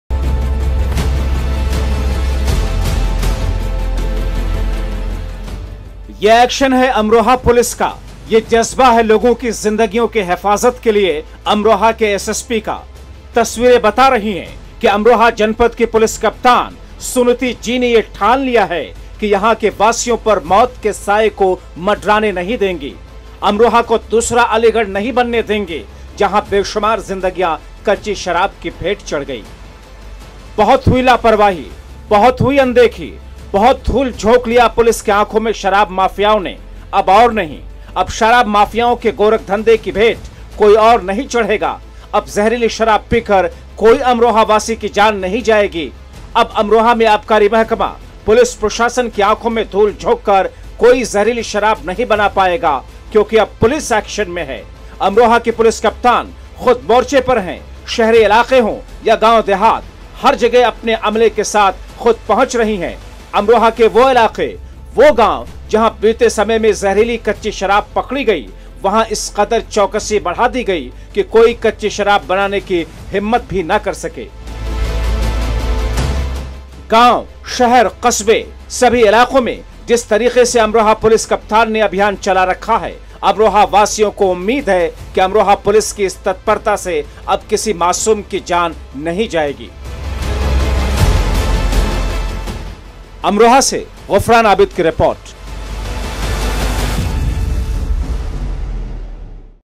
शराब माफियाओं के ख़िलाफ़ अमरोहा पुलिस का महाभियान।
अमरोहा की खबरें  : अब नहीं जाएगी किसी मासूम की जानअमरोहा पुलिस ने ठाना-शराब माफियाओं को नहीं मिलेगा ठिकाना
हर बड़ी ख़बर पर रहेगी  अमरोहा TV की नजर...नहीं छूटेगी कोई भी ख़बर...हर ख़बर से हम आपको कराएंगे बाखबर...देखते रहिए अमरोहा TV